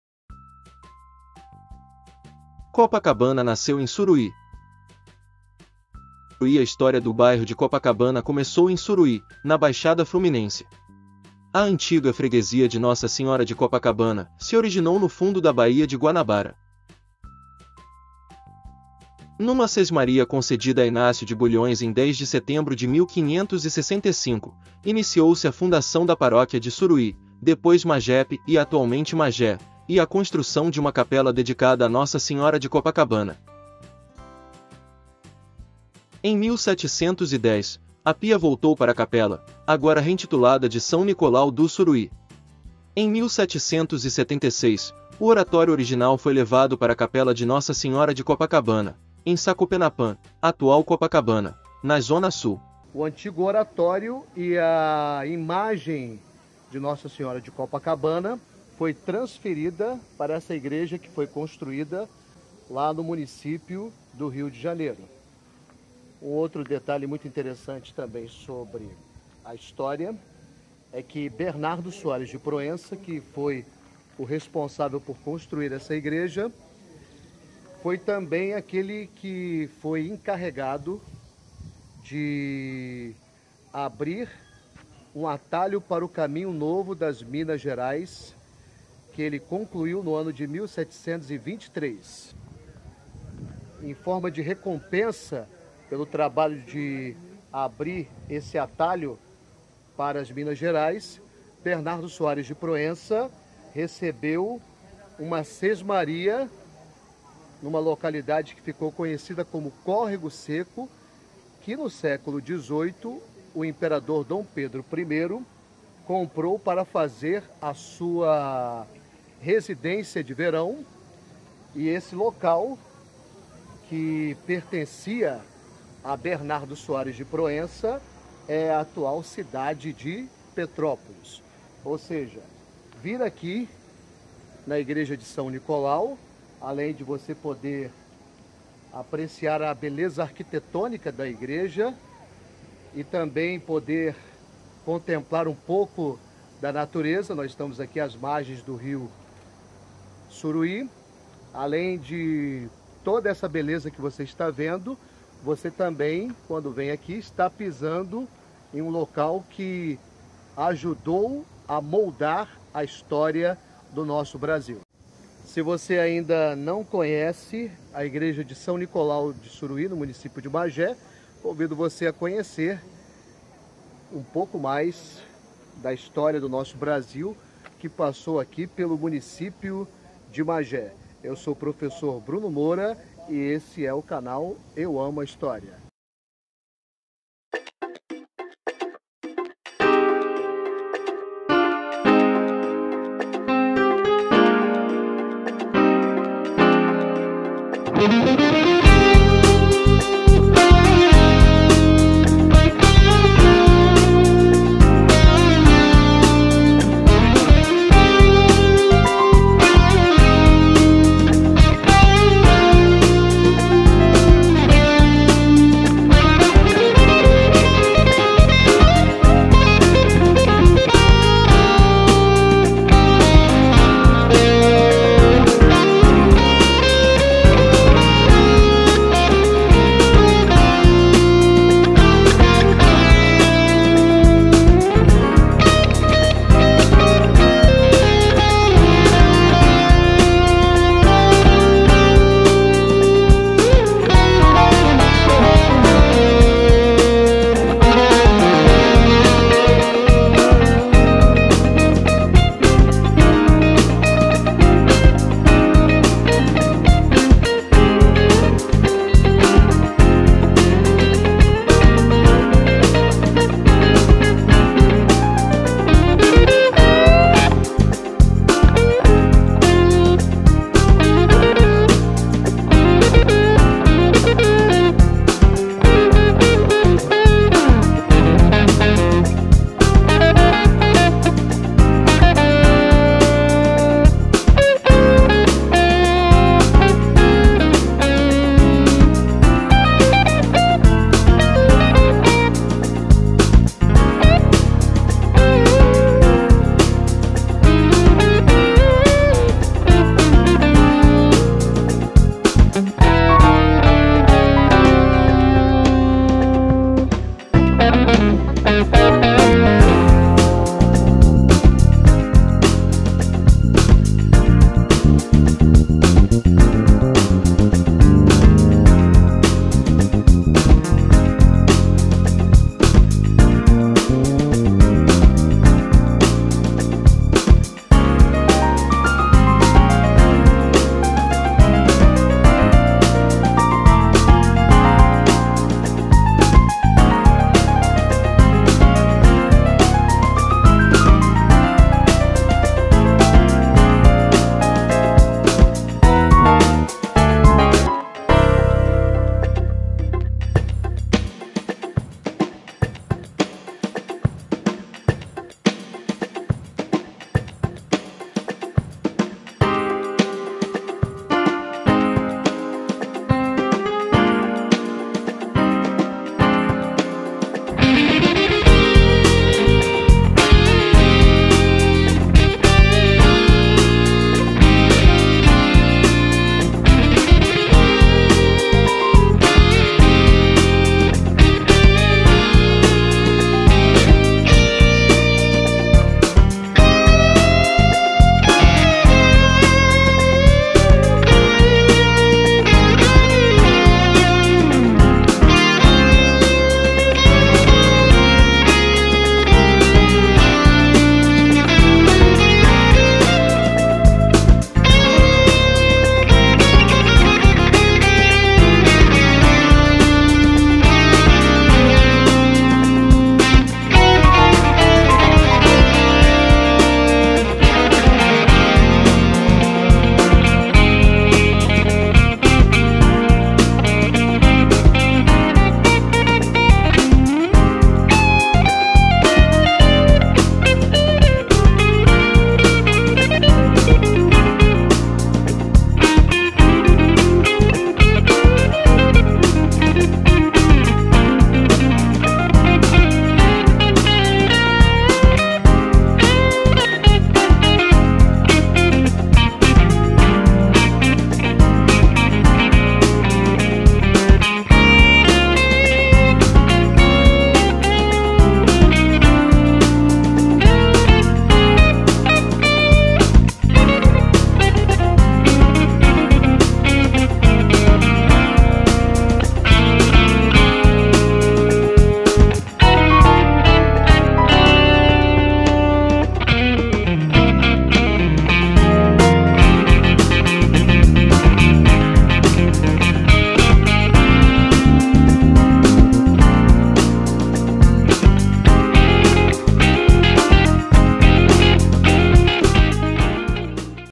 Copacabana nasceu em Suruí .
A história de Copacabana
Copacabana é um bairro situado na Zona Sul do município do Rio de Janeiro, no Brasil. É considerado um dos bairros mais famosos e prestigiados do Brasil e um dos mais conhecidos do mundo. Tem o apelido de Princesinha do Mar e Coração da Zona Sul. Faz limites com os bairros da Lagoa, Ipanema, Botafogo, Leme e Humaitá.
Etimologia
Há várias hipóteses etimológicas para o nome Copacabana. A primeira alega que o termo teria vindo da língua quíchua falado no antigo Império Inca, significando "lugar luminoso", "praia azul" ou "mirante do azul". Outras fontes apontam o termo como originário da língua aimará falada na Bolívia, significando "vista do lago" (kota kahuana). Nesse país, Copacabana é o nome dado a uma cidade situada às margens do Lago Titicaca, fundada sobre um antigo local de culto inca. Existem relatos de que, nesse local, antes da chegada dos colonizadores espanhóis, ocorria o culto a uma divindade chamada Kopakawana, que protegeria o casamento e a fertilidade das mulheres.
A lenda de Nossa Senhora de Copacabana
Segundo uma lenda, após a chegada dos espanhóis à região da Copacabana boliviana, Nossa Senhora teria aparecido no local para Francisco Tito Yupanqui, um jovem pescador, que, em sua homenagem, teria esculpido uma imagem da santa que ficou conhecida como Nossa Senhora de Copacabana: a Virgem vestida de dourado pousada sobre uma meia-lua.
O antiga praia de Sacopenapã
Inicialmente, a praia e toda a região em volta do atual bairro de Copacabana tinham o nome tupi de "Sacopenapã", que significa "o barulho e o bater de asas dos socós".

Copacabana nasce em Suruí (Na baixada Flumimnense)
A história do atual bairro de Copacabana começou em Suruí (na Baixada Fluminense ) na antiga da "Freguesia de Nossa Senhora de Copacabana" ,no fundo da Baía de Guanabara.

Numa sesmaria do recôncavo da Baía de Guanabara, concedida a Inácio de Bulhões em 10 de Setembro de 1565, iniciou-se a fundação da Paróquia de Suruí (depois Magepe e atualmente Magé) e a construção de uma capela dedicada a Nossa Senhora de Copacabana. Com o passar do tempo, as paredes ficaram em ruínas e a pia batismal foi então transferida para uma pequena ermida próxima, construída em 1628.

Em 1710, a pia voltou para a capela, agora reintitulada de São Nicolau do Suruí e reconstruída de forma mais resistente, com paredes de pedra e cal. Em 1776, o oratório original foi levado para a Capela de Nossa Senhora de Copacabana, em Sacopenapã (atual Copacabana, na Zona Sul), que acabara de ser construída em um rochedo no final da praia. Com a transferência do oratório e da imagem de Nossa Senhora de Copacabana de Suruí para Sacopenapã ,  o nome da praia e da região foi trocado para "Copacabana"..